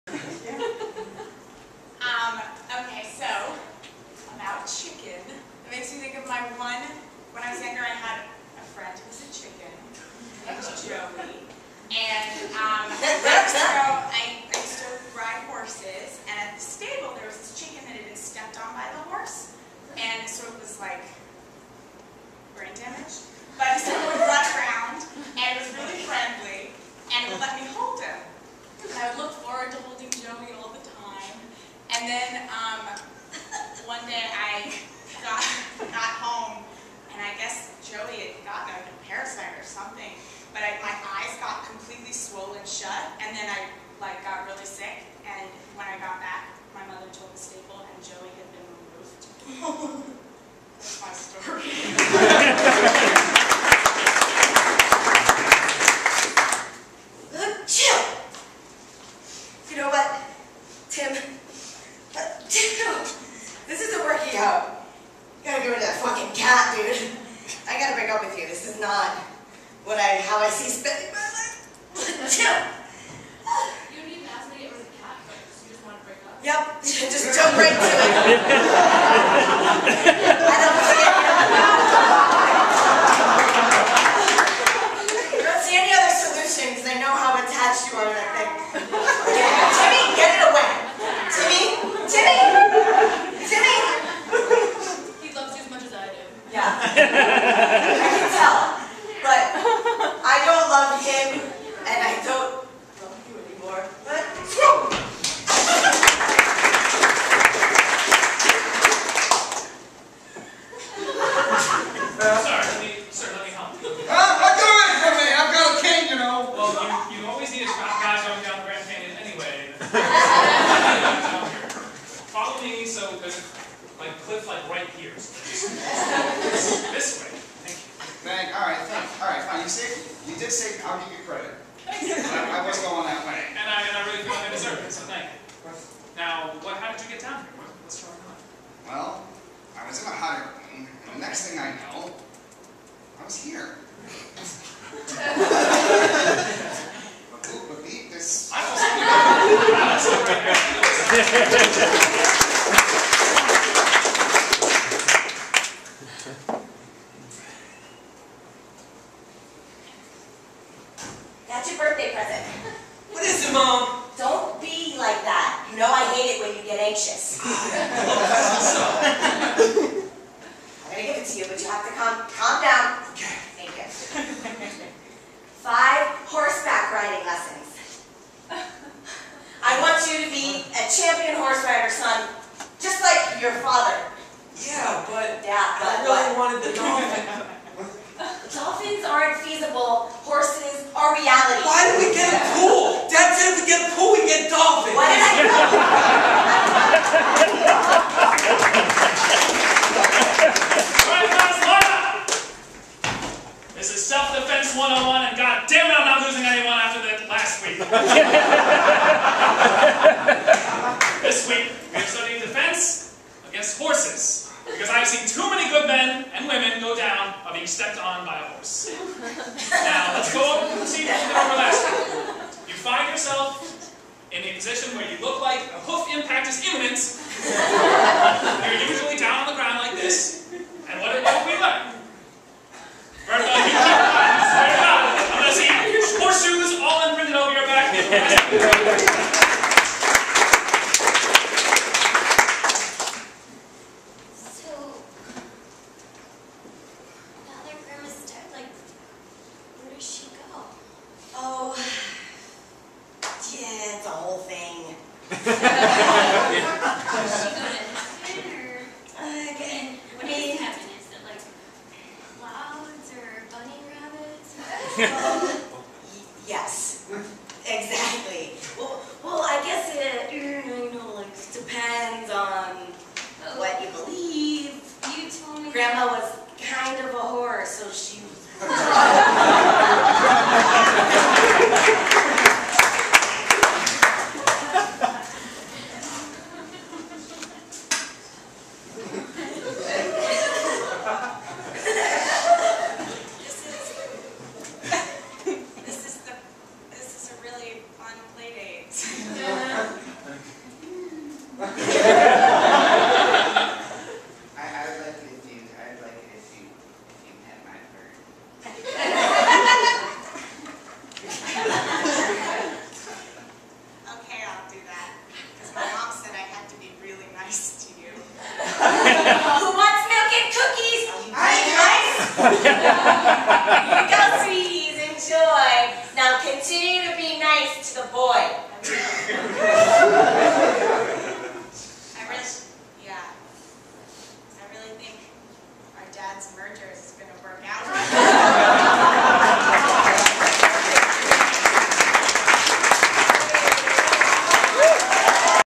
Magnet Theater: Level 1 Class Show [GROUP 1: Part C]
Magnet Theater: Level 1 Class Show (10.08.10). Directed by: Mark Grenier

GROUP 1 PLAYERS: Beth Gold, Jason Hoskins, Jerry Lenihan, Annie Paul, Michael Petrocelli, Hannah Roshetko